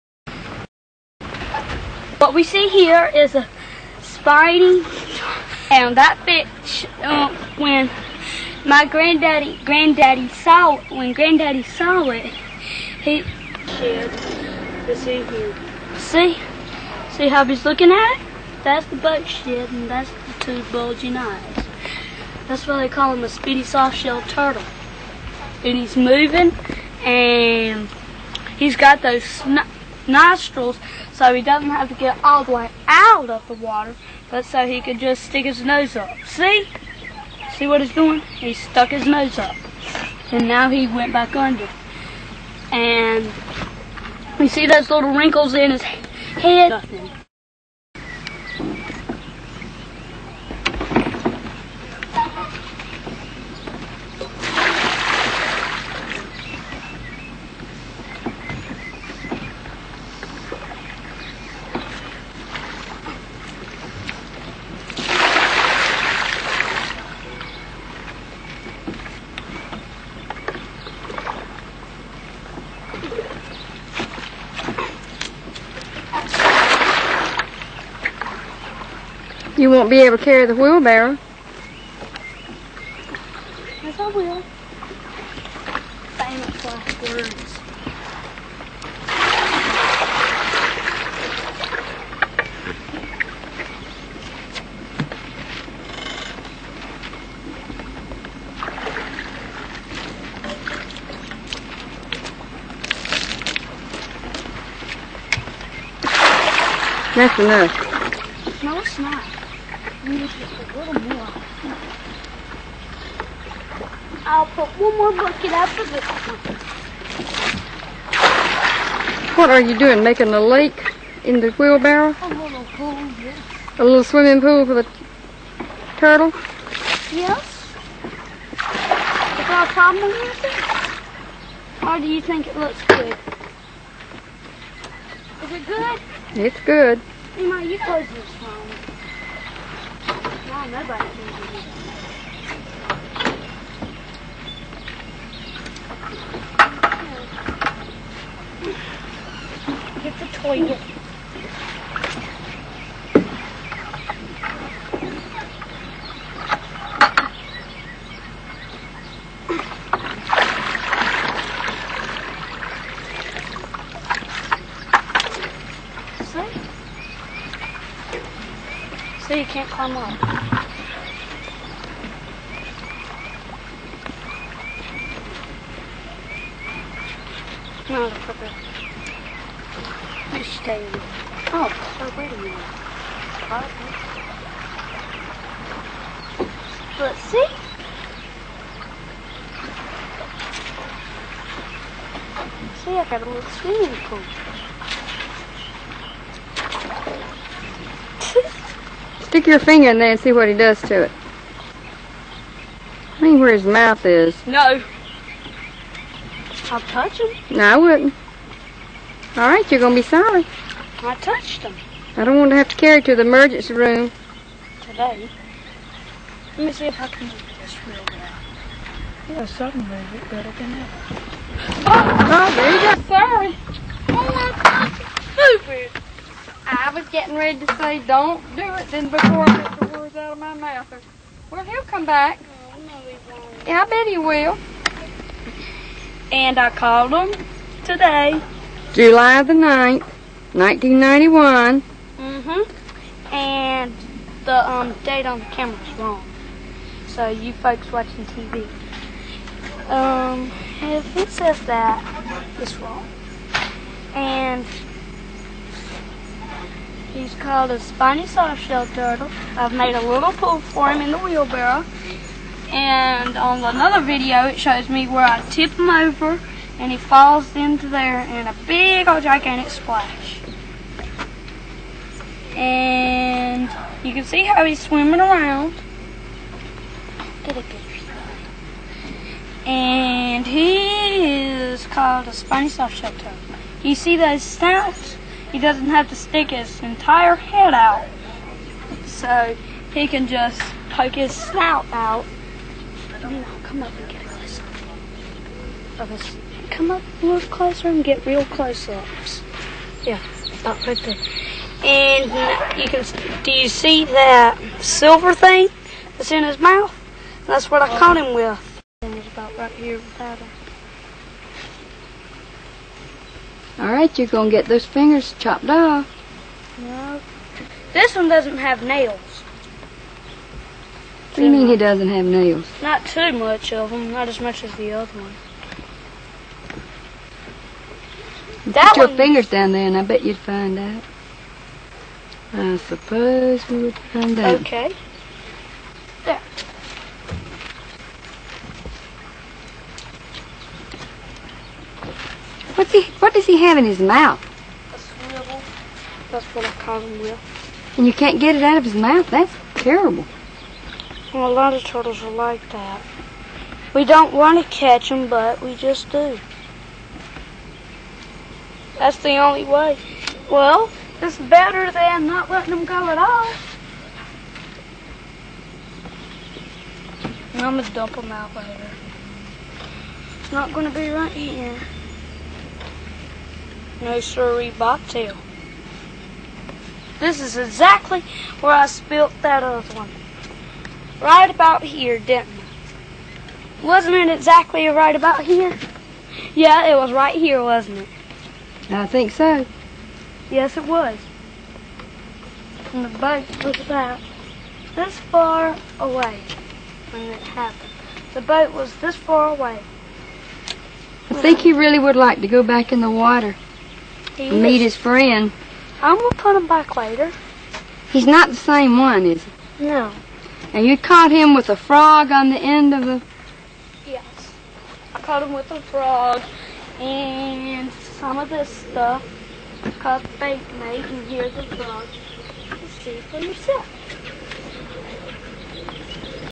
You Don't Know As Much About Turtles As I Do - Part 2
Mikey continues his dissertation on the "Spinny" softshell turtle.